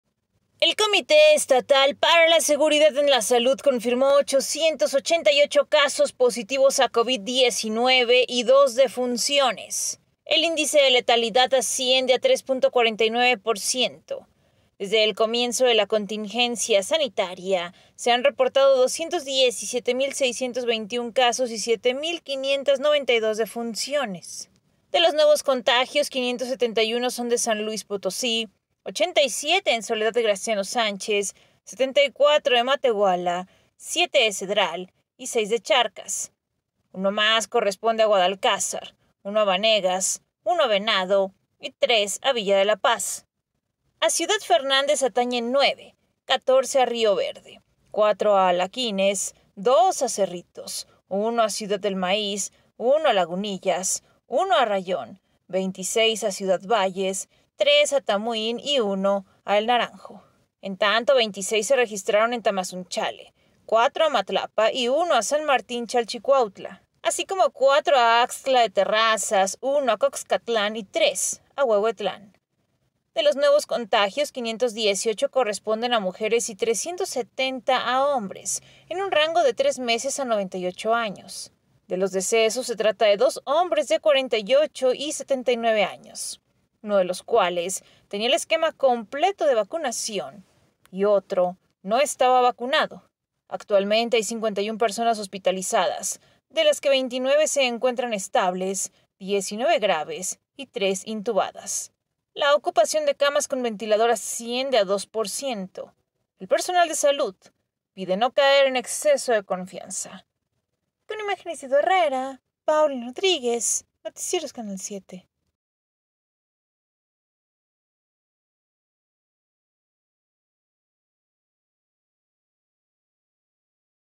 Hospitalizaciones por COVID-19 escalan a 51 en SLP, donde se registraron 888 nuevos contagios y...
Hospitalizaciones por COVID-19 escalan a 51 en SLP, donde se registraron 888 nuevos contagios y dos defunciones.

SLP.- El Comité Estatal para la Seguridad en la Salud confirmó 888 casos positivos a COVID-19 y dos defunciones.

El índice de letalidad asciende a 3.49 por ciento.

Desde el comienzo de la contingencia sanitaria se han reportado 217 mil 621 casos y siete mil 592 defunciones.

De los nuevos contagios, 571 son de San Luis Potosí, 87 en Soledad de Graciano Sánchez, 74 de Matehuala, siete de Cedral y seis de Charcas.

Uno más corresponde a Guadalcázar, uno a Vanegas, uno a Venado y tres a Villa de la Paz.

A Ciudad Fernández atañen nueve, 14 a Rioverde, cuatro a Alaquines, dos a Cerritos, uno a Ciudad del Maíz, uno a Lagunillas, uno a Rayón, 26 a Ciudad Valles, tres a Tamuín y uno a El Naranjo.

En tanto, 26 se registraron en Tamazunchale, cuatro a Matlapa y uno a San Martín Chalchicuautla.

Así como cuatro a Axtla de Terrazas, uno a Coxcatlán y tres a Huehuetlán.

De los nuevos contagios, 518 corresponden a mujeres y 370 a hombres; en un rango de tres meses a 98 años.

De los decesos, se trata de dos hombres de 48 y 79 años.

Uno de los cuales tenía el esquema completo de vacunación y otro no estaba vacunado.

Actualmente hay 51 personas hospitalizadas, de las cuales 29 se encuentran estables, 19 graves y tres intubadas.

La ocupación de camas con ventilador asciende a dos por ciento.

El personal de salud pide no caer en exceso de confianza.